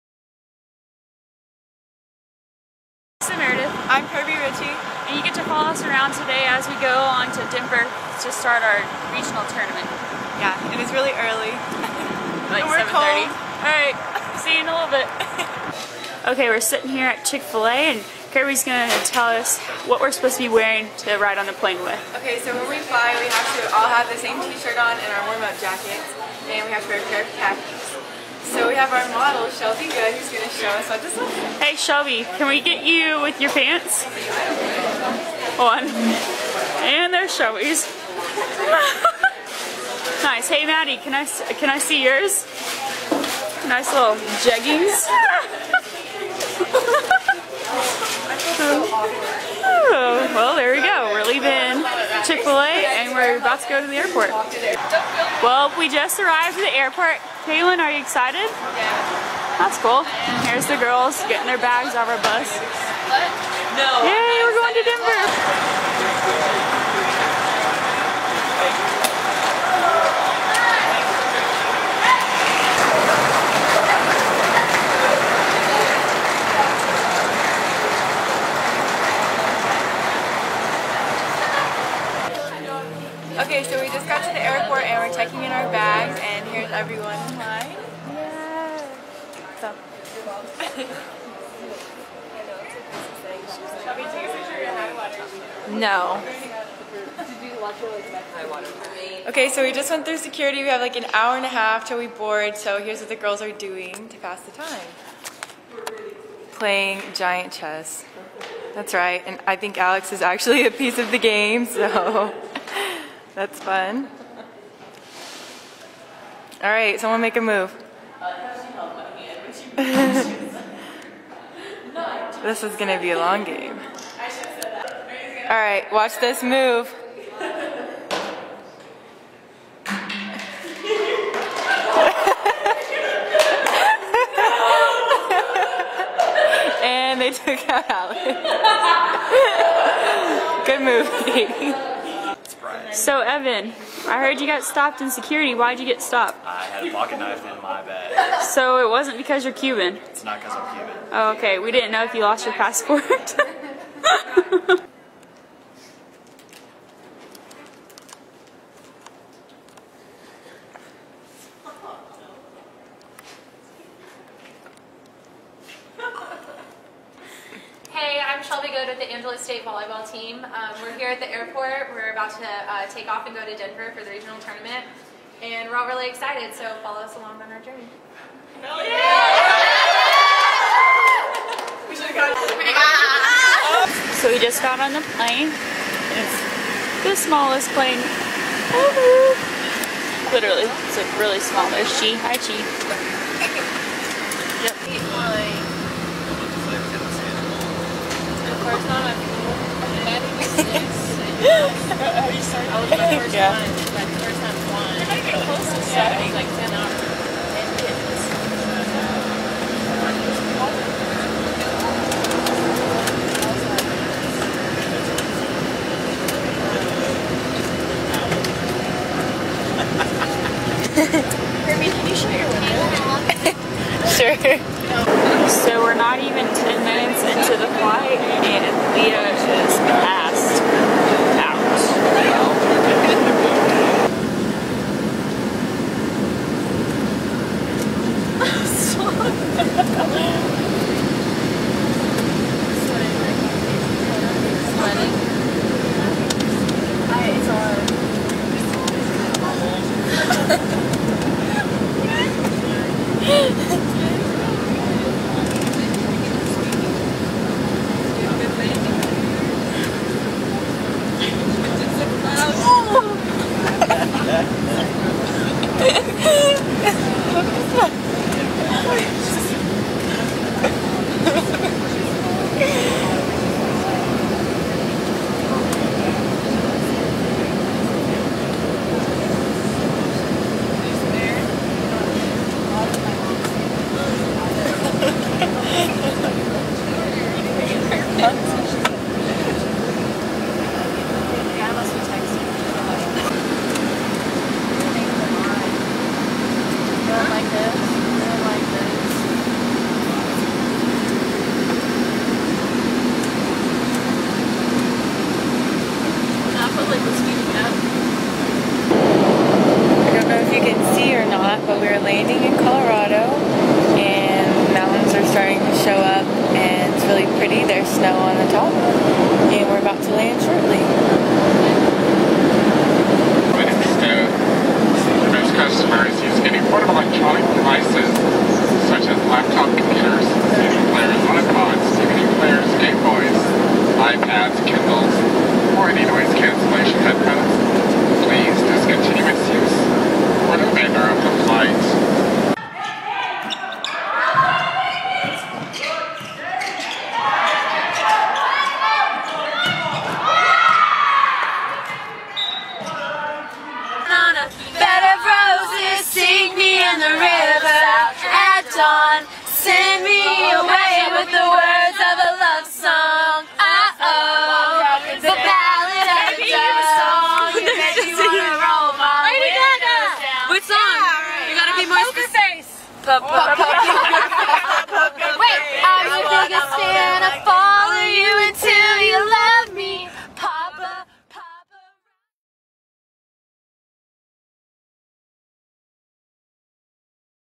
Angelo State Volleyball (11.27.2012)
From San Angelo to Lubbock and then off to Denver, the Angelo State volleyball has made it safely to the site of this week's NCAA DII South Central Regional Championships. We take you along for the rides that got them here in this video. The Belles will open the tournament at 5 p.m. (MT) on Thursday against Adams State at the Regis Fieldhouse.